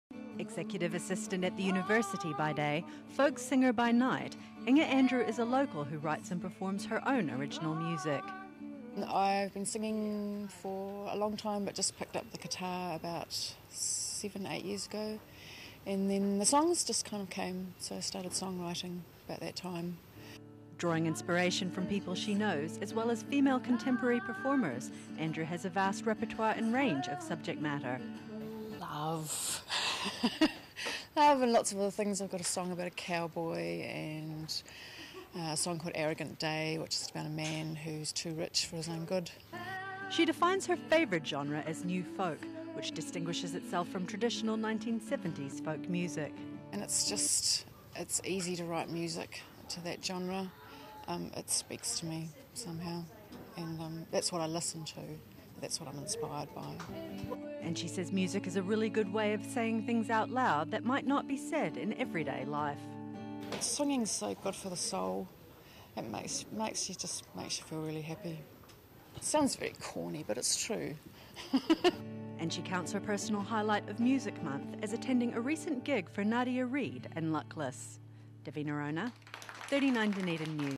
Guitarist Inge Andrew performs folk music in the Library
As part of Music Month, singer and guitarist Inge Andrew performed folk music in the library today. And she writes songs from the heart about love, loss, spoilt rich men - and the occasional cowboy.